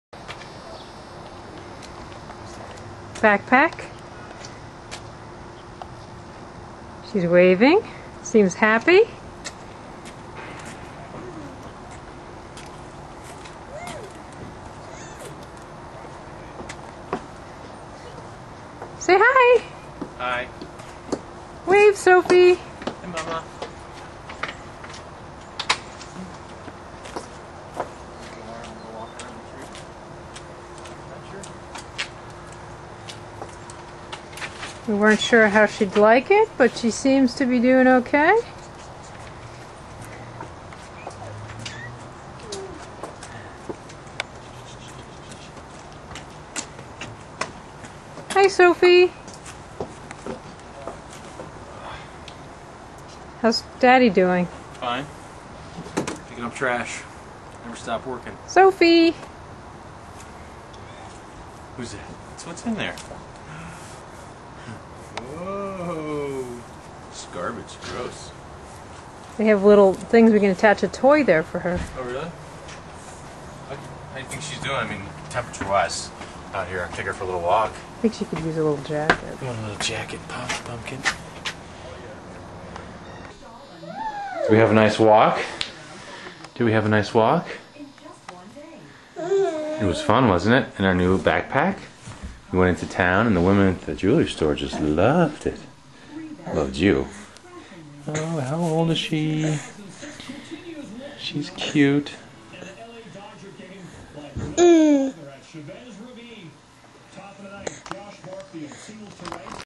New Backpack
sophie  gets her first ride on the back